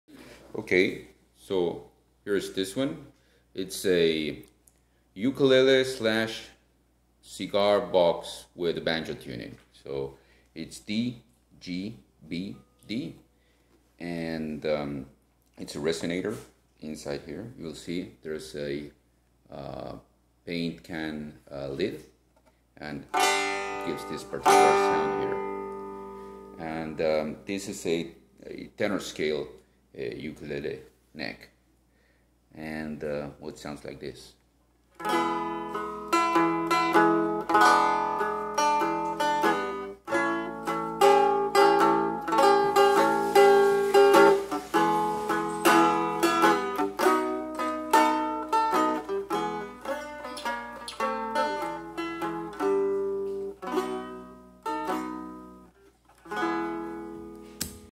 Cigar Box Ukulele guitar sound sample
so this video is a sound sample of this handmade cigar box ukulele. it is tuned to open g same as in evangel and it has a resonator plate made out from a paint lid can